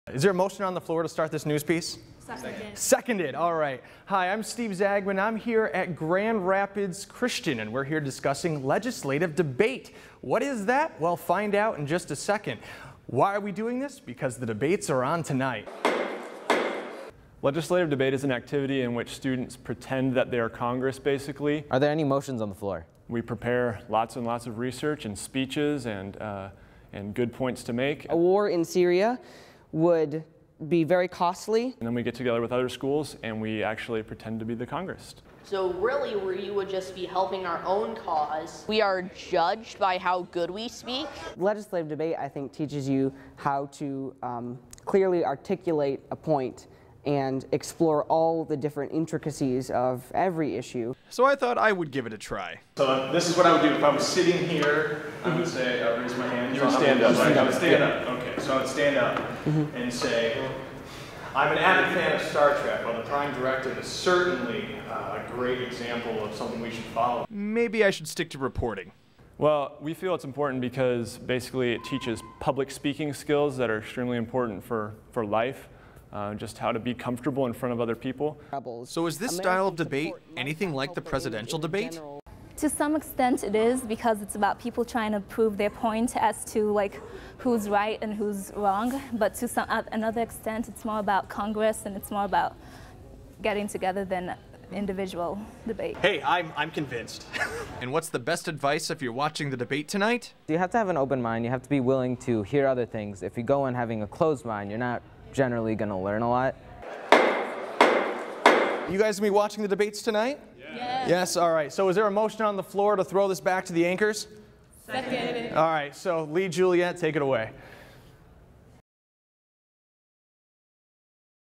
Steve learns how to debate again - From President to High School Debate
The world of debate is fun.  I know this because i use to do this back in the day, but in this video i learn about a different form of debate called legislative debate.